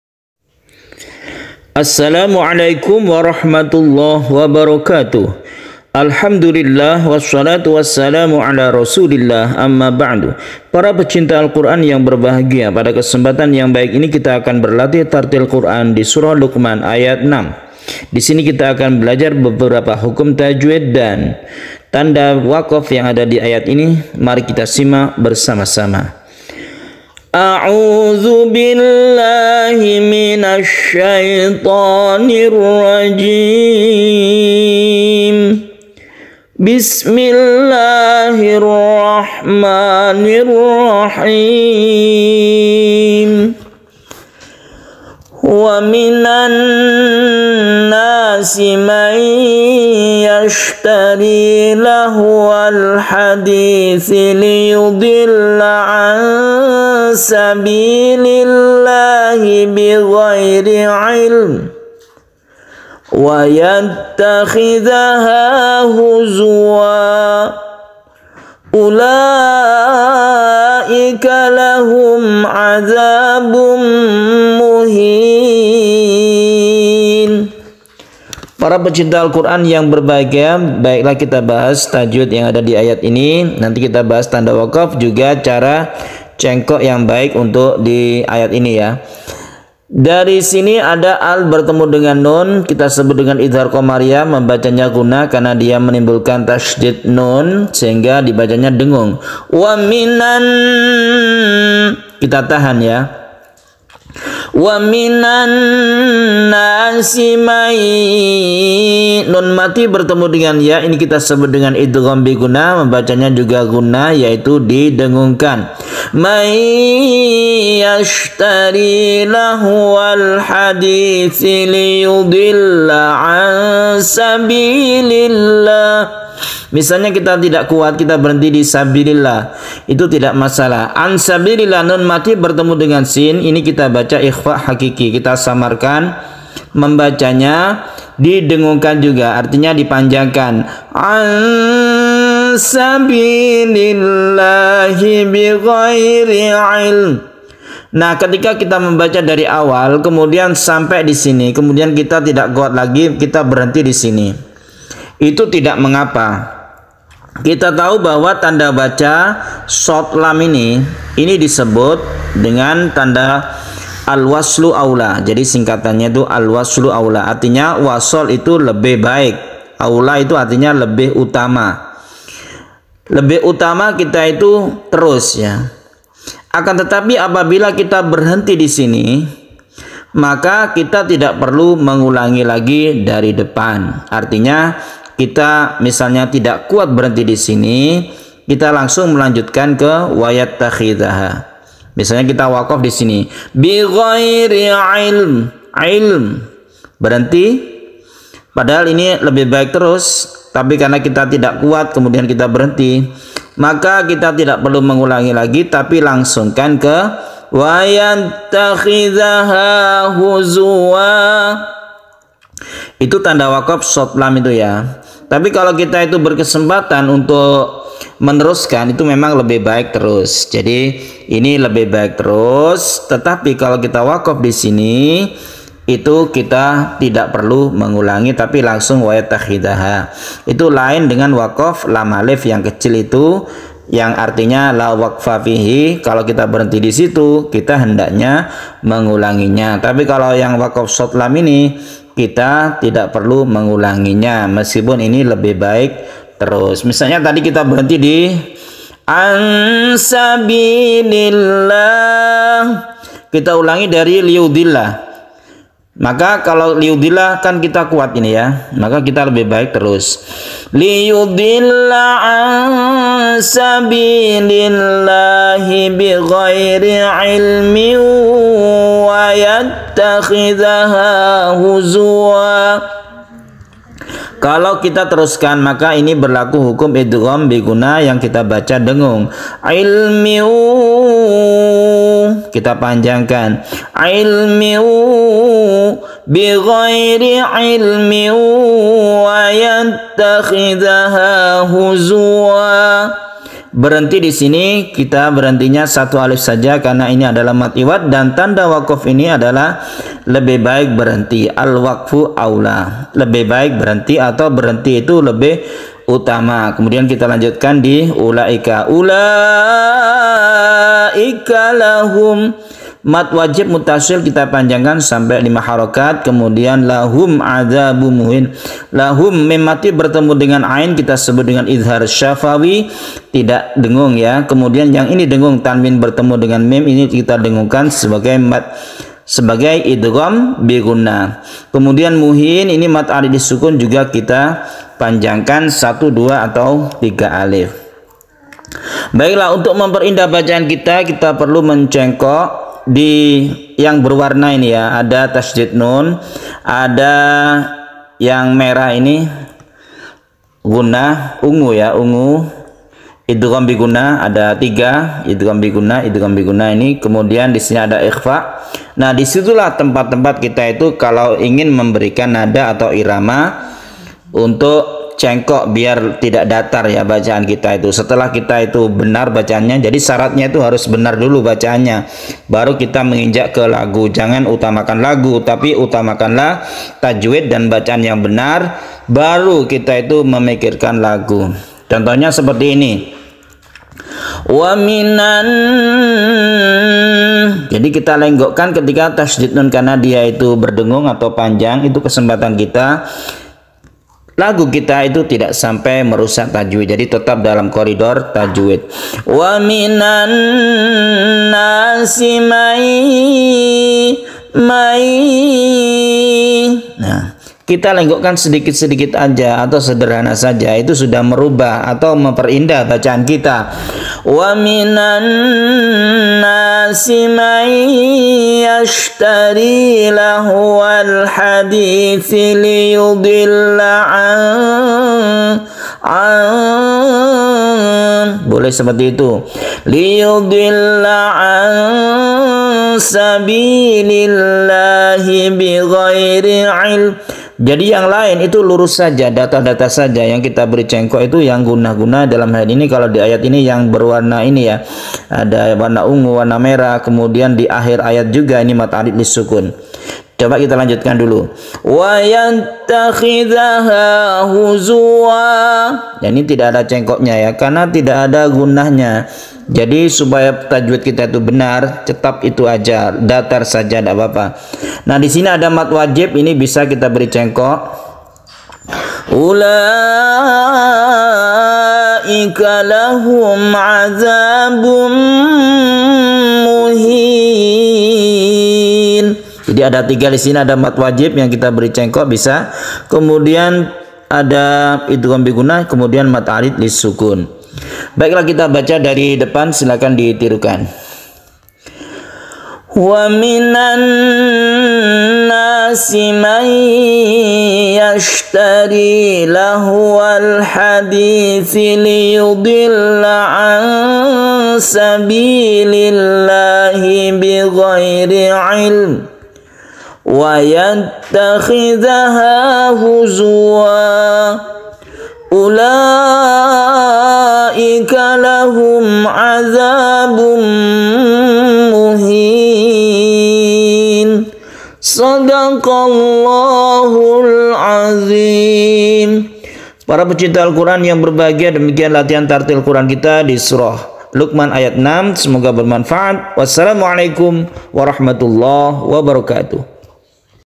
Latihan Cengkok dengan Memahami Tajwid dan Tanda Waqaf Surat Luqman 6
Fasih Baca Quran dengan latihan tartil Quran sesuai Tajwid